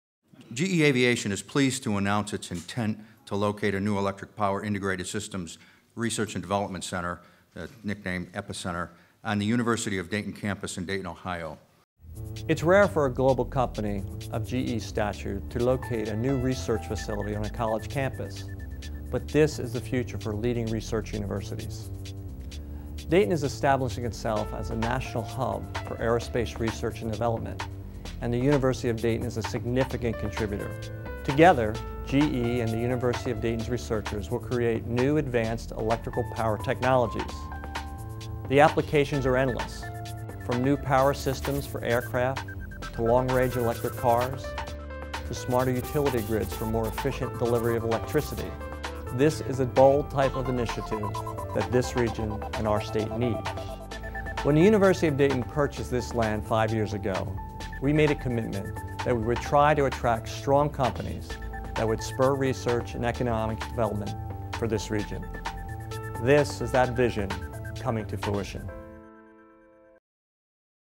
GE Announces EPISCENTER at the University of Dayton
GE Aviation announced today that it's building a $51 million research facility on the University of Dayton's campus, what President Daniel J. Curran calls "the wave of the future for leading research universities."